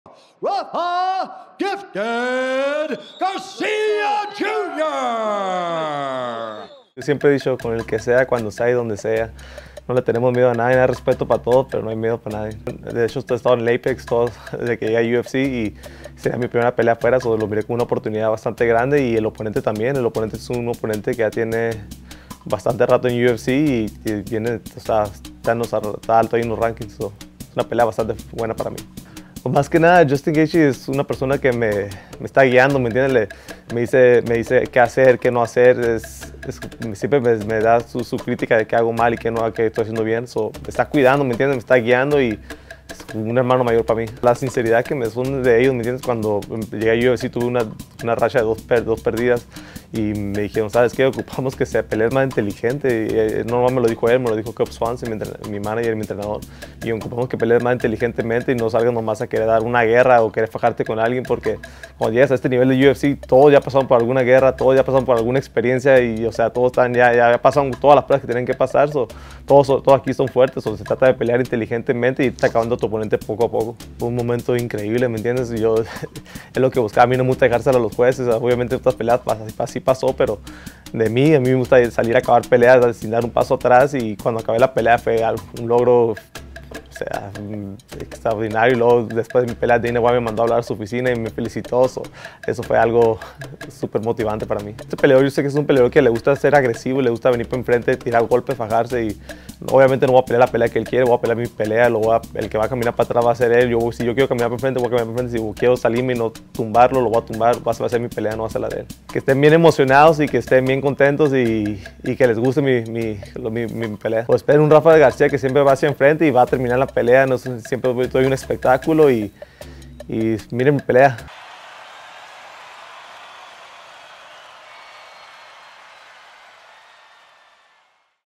UFC277 Rafa García: A Continuar Racha Ganadora
El peleador mexicano Rafa García tiene un importante compromiso en el octágono el Sáb. 30 de julio en UFC277. Esto comentó de su pelea.